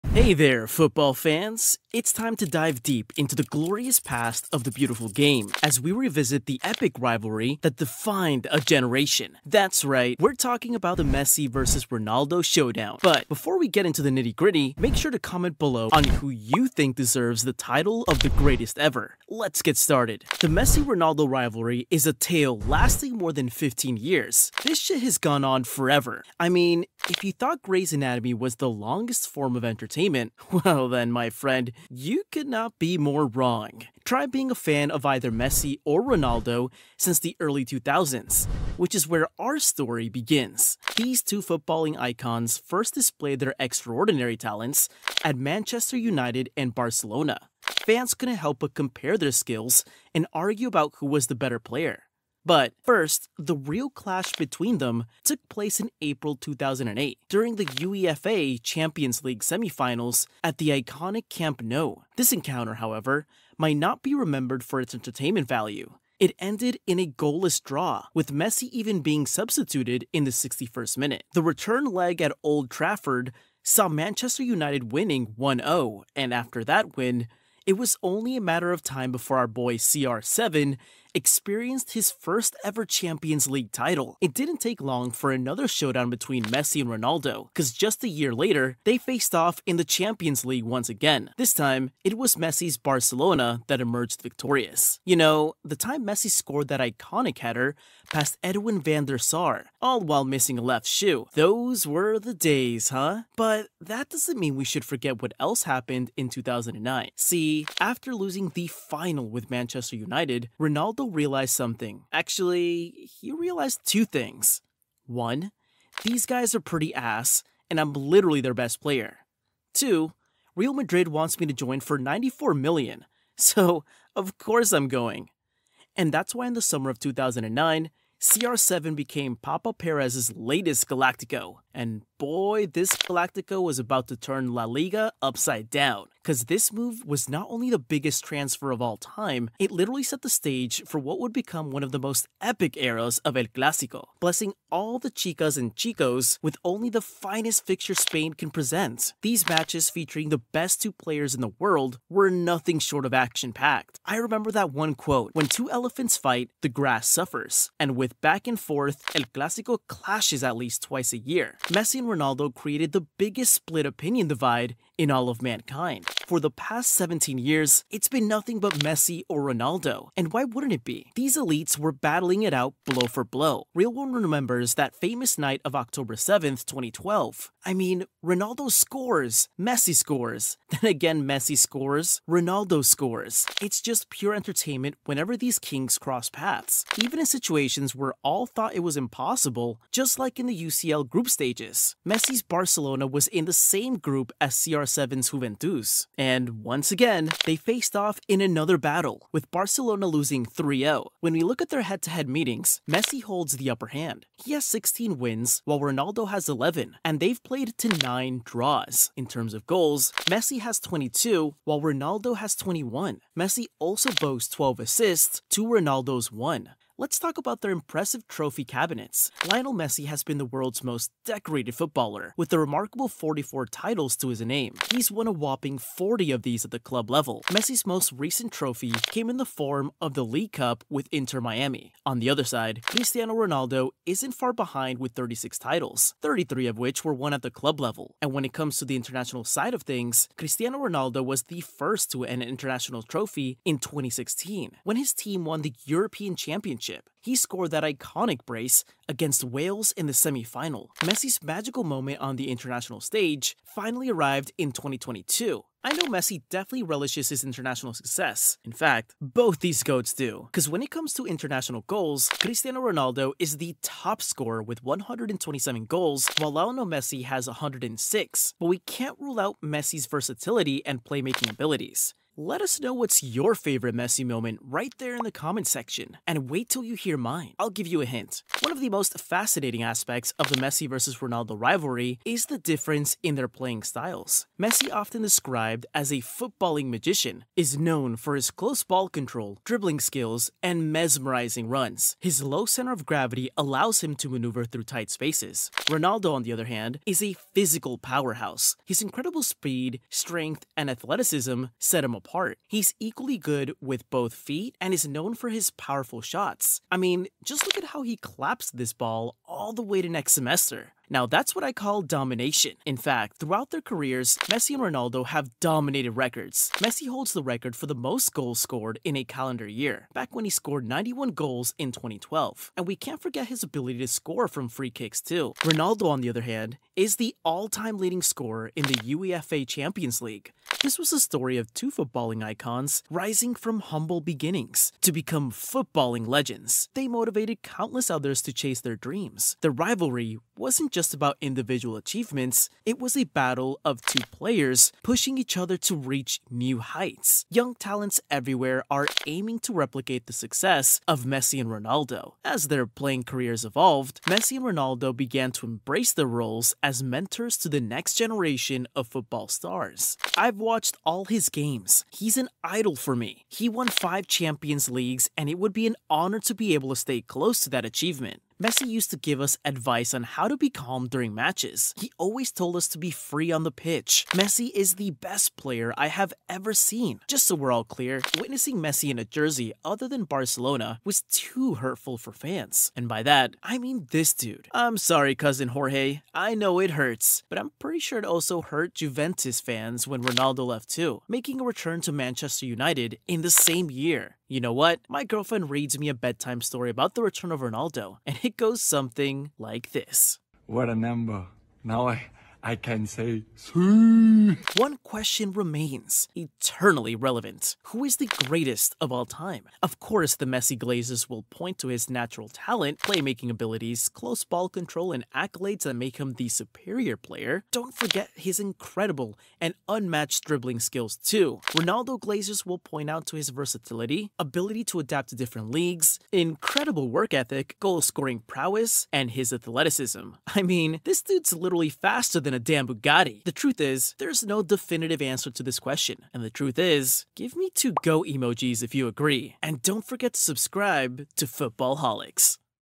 Legendary Rivalry Of Messi And Ronaldo!
Dive into the world of football as we explore the legendary 15 year rivalry between Messi and Ronaldo! This epic showdown has captivated fans and reshaped the landscape of soccer, pitting two of the greatest players against each other in a battle for supremacy. From unforgettable goals to iconic moments on the pitch, this video covers the highs and lows of their storied careers. Join us as we analyze their incredible statistics, memorable matches, and the impact they've had on the sport. Whether you're a die-hard fan or new to the game, this deep dive into the Messi vs Ronaldo rivalry will leave you in awe of their talent and dedication. Don't miss out on this celebration of football greatness!